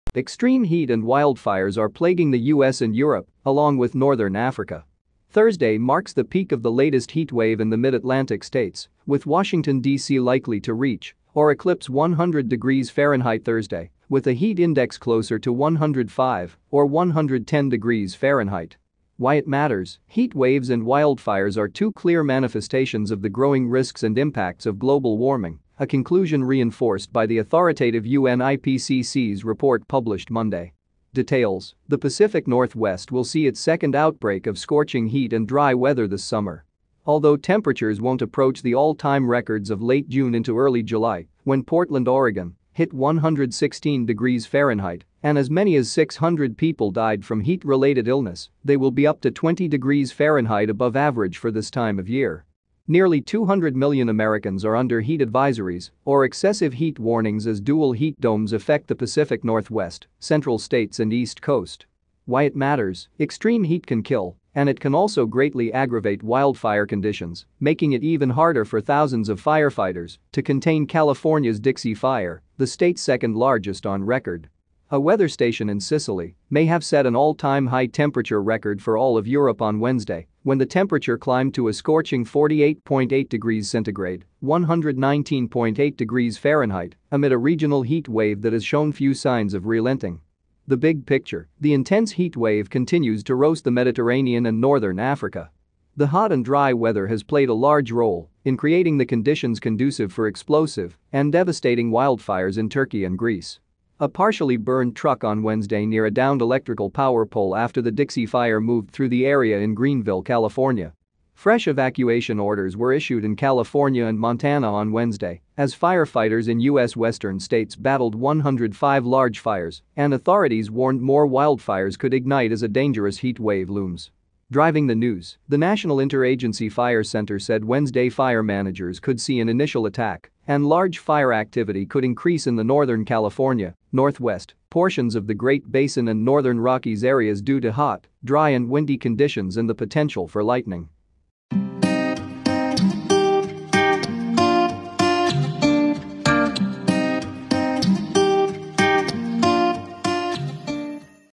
Weather map of Europe showing hot temperatures represented in red.
Extreme heat and wildfires are plaguing the U.S. and Europe, along with northern Africa. Thursday marks the peak of the latest heat wave in the Mid-Atlantic states, with Washington, D.C. likely to reach or eclipse 100°F Thursday, with a heat index closer to 105 or 110°F. Why it matters: Heat waves and wildfires are two clear manifestations of the growing risks and impacts of global warming, a conclusion reinforced by the authoritative U.N. IPCC's report published Monday. Details: The Pacific Northwest will see its second outbreak of scorching heat and dry weather this summer. Although temperatures won't approach the all-time records of late June into early July, when Portland, Ore., hit 116°F, and as many as 600 people died from heat-related illness, they will be up to 20°F above average for this time of year. Nearly 200 million Americans are under heat advisories or excessive heat warnings as dual "heat domes" affect the Pacific Northwest, Central states and East Coast. Why it matters: Extreme heat can kill, and it can also greatly aggravate wildfire conditions, making it even harder for thousands of firefighters to contain California's Dixie Fire, the state's second-largest on record. A weather station in Sicily may have set an all-time high temperature record for all of Europe on Wednesday, when the temperature climbed to a scorching 48.8°C (119.8°F) amid a regional heat wave that has shown few signs of relenting. The big picture: The intense heat wave continues to roast the Mediterranean and northern Africa. The hot and dry weather has played a large role in creating the conditions conducive for explosive and devastating wildfires in Turkey and Greece. A partially burned truck on Wednesday near a downed electrical power pole after the Dixie Fire moved through the area in Greenville, California.  Fresh evacuation orders were issued in California and Montana on Wednesday, as firefighters in U.S. Western states battled 105 large fires — and authorities warned more wildfires could ignite as a dangerous heat wave looms. Driving the news: The National Interagency Fire Center said Wednesday fire managers could see an "[i]nitial attack and large fire activity could increase in the Northern California, Northwest, portions of the Great Basin and Northern Rockies areas due to hot, dry and windy conditions and the potential for lightning."